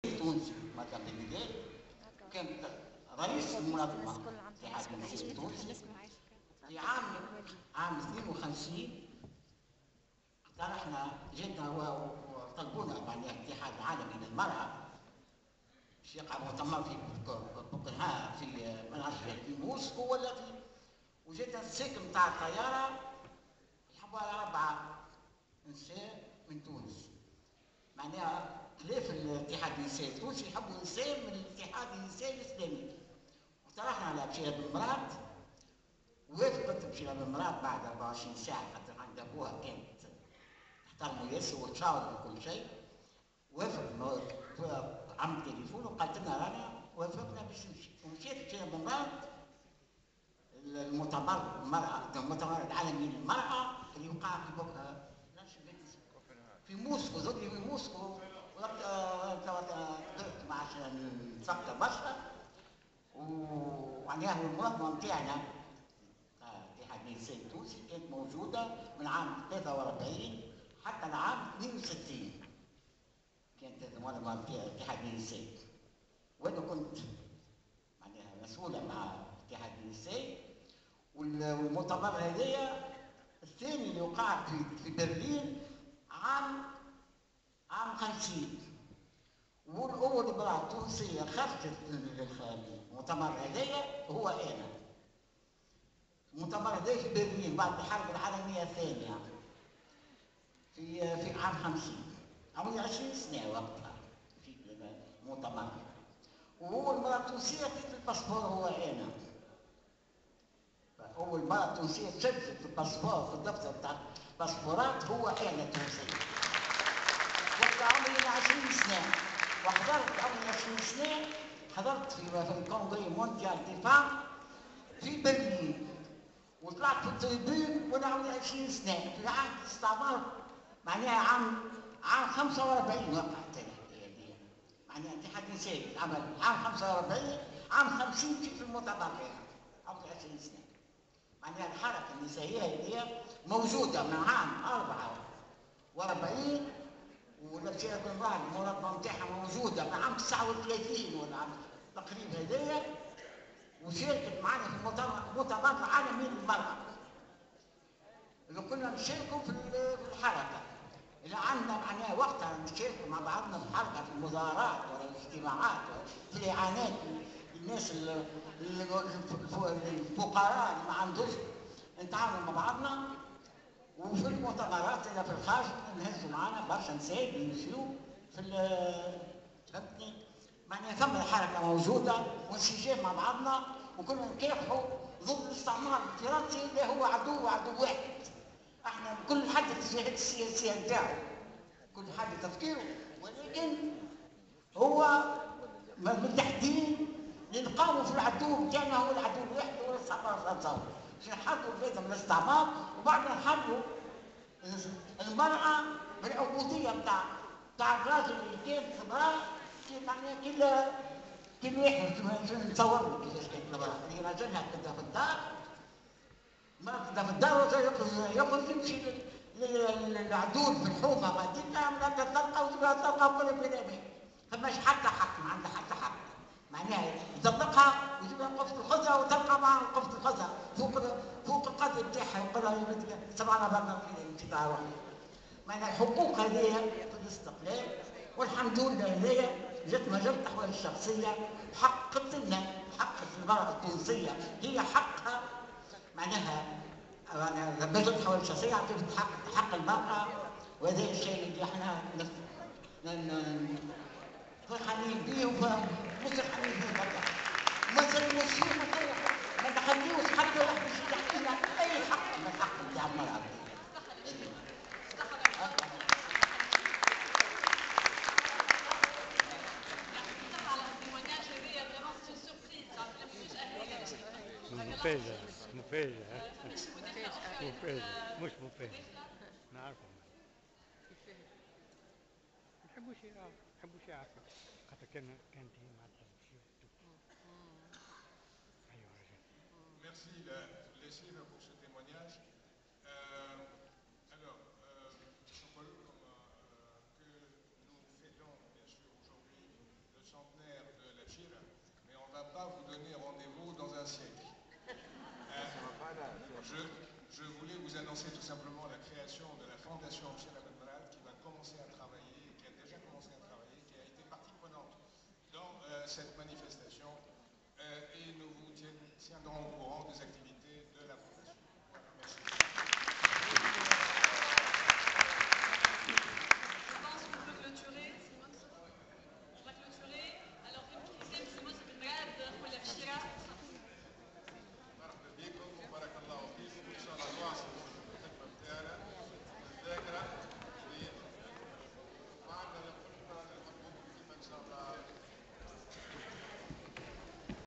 Chérifa Saadaoui, militante des années 40-50-60 parle de la lutte des femmes tunisiennes
Chérifa Saadaoui, militante des années 40-50-60, secrétaire générale de ittihad nissa tounes fait un témoignage durant la commémoration du centenaire de la militante bchira ben mrad le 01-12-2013 au théâtre municipal.